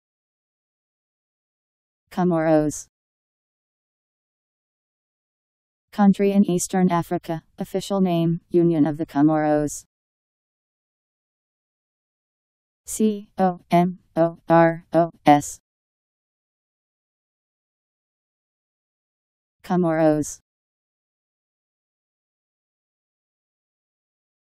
Comoros Meaning
Video shows what Comoros means. Country in Eastern Africa. Official name: Union of the Comoros..  Comoros pronunciation. How to pronounce, definition by Wiktionary dictionary. Comoros meaning. Powered by MaryTTS